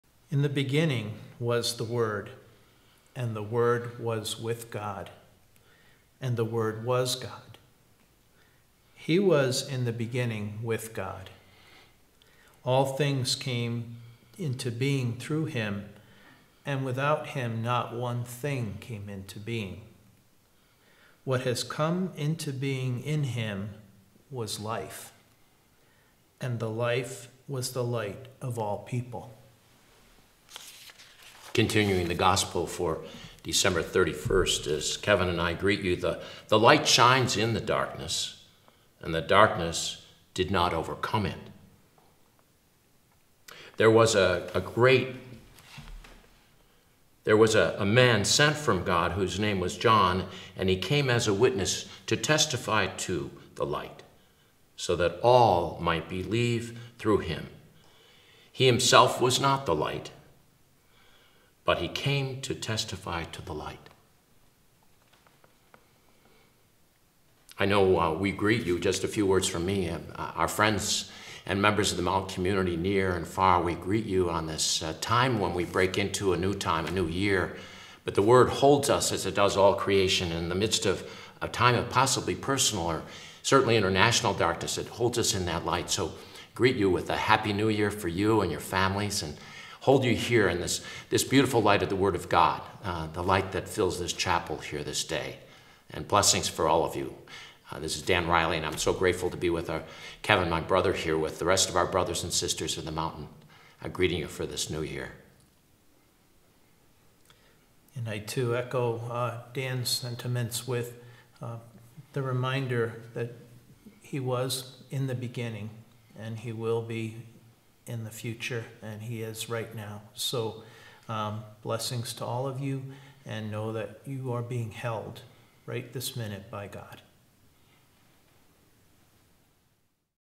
2016 New Year's Video Greeting from Mt. Irenaeus
A new year's greeting from Fr. Dan Riley, ofm, and Br. Kevin Kriso, ofm, of Mt. Irenaeus.